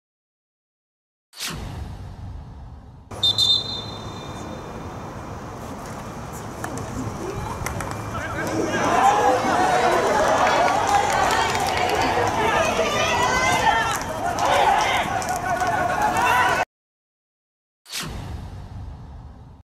【Ｈ１】一番騎がシュートブロックのスーパープレー！山梨学院OCTOBER EAGLESのＦＢ２３田辺 直也がＰＣを止めた！
第５日
立命館ホリーズ vs 山梨学院 OCTOBER EAGLES
＠立命館ホリーズスタジアム